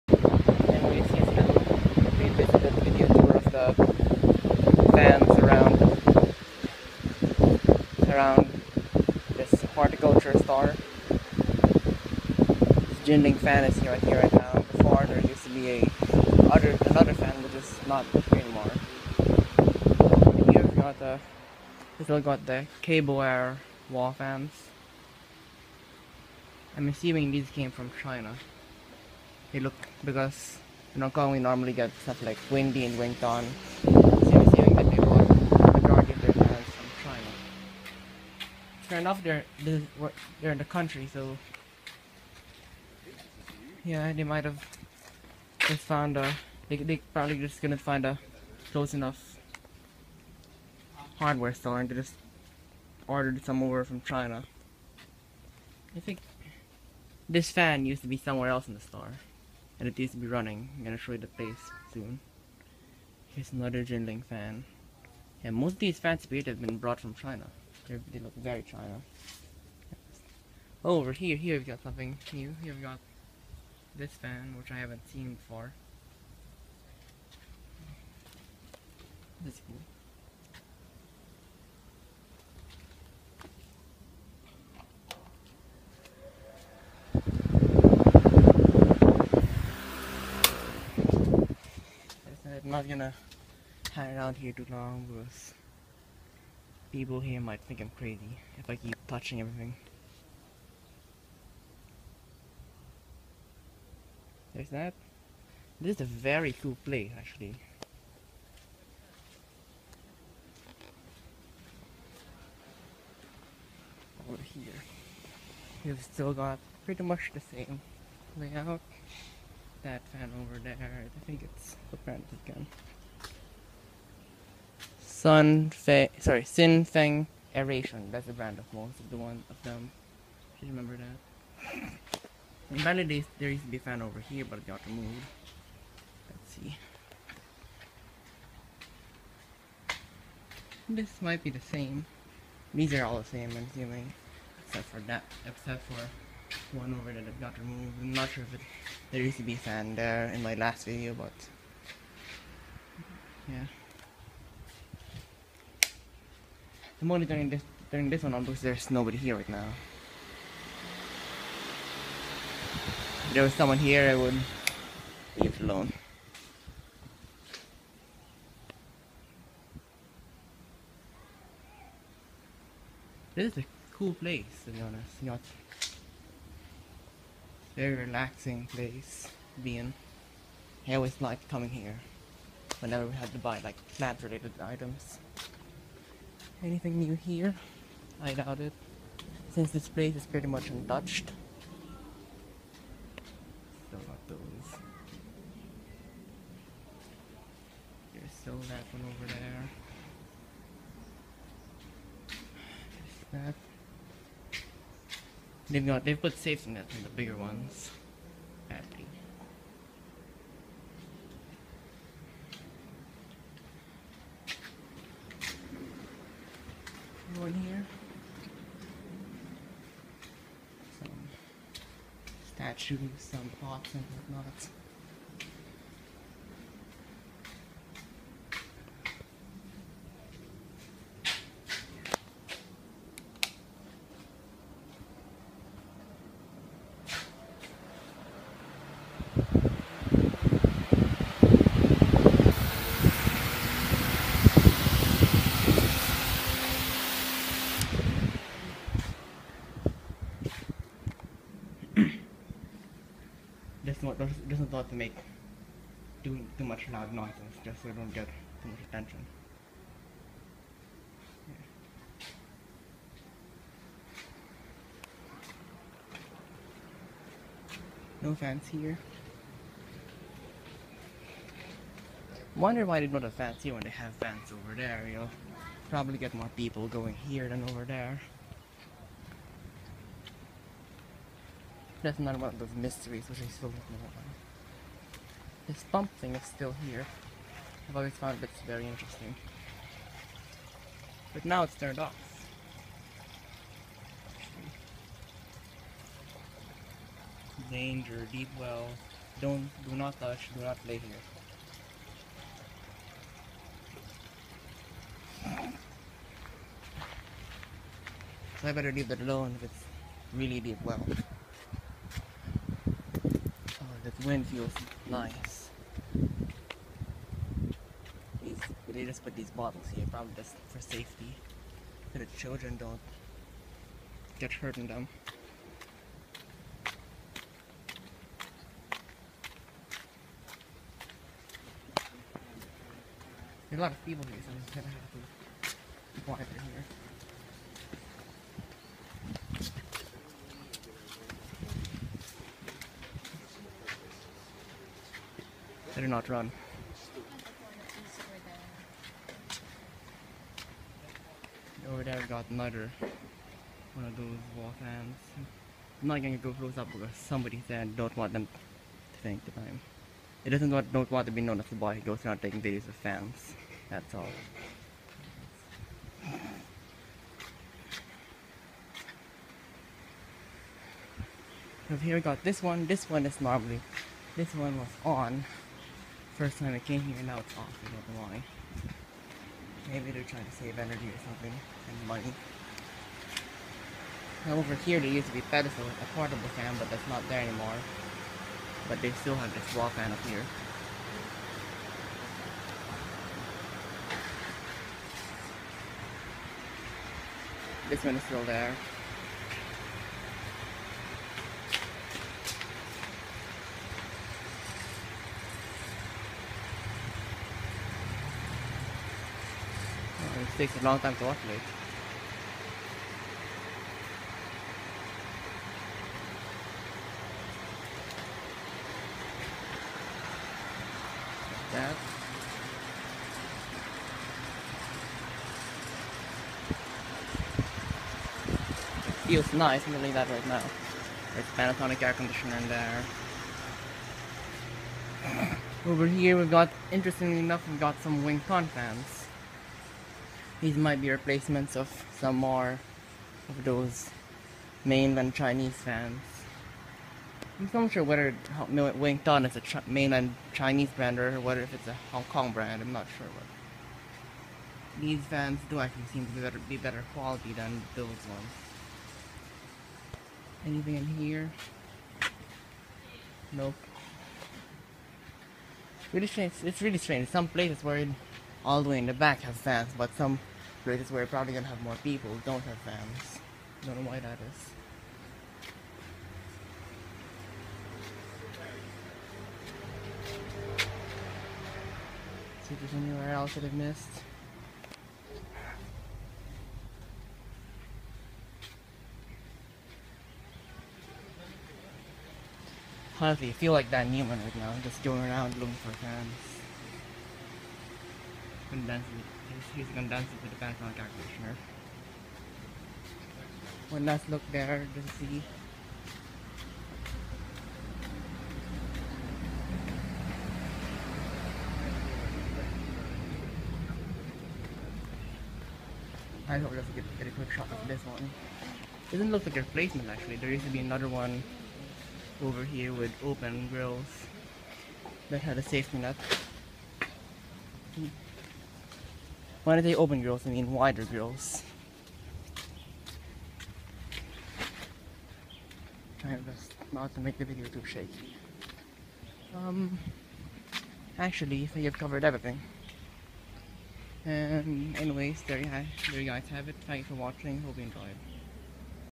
(Revisited 2016) Video tour of wall fans and portable fans at a garden center
Some slight changes, but everything else is pretty much the same.

Most fans have a blade diameter of around 20" (51cm). Some are bigger, with a blade diameter of around two feet (24" (61cm)).

Brands shown:
- Jinling
- Pufeng
- Xin Feng Aeration
- Kaiboer (if that's how it's spelled)
- Wing Ton

Tags: front back guard cage grill grille metal high velocity capacity industrial commercial duty horticulture agriculture HVAC cooling air conditioning aircon vac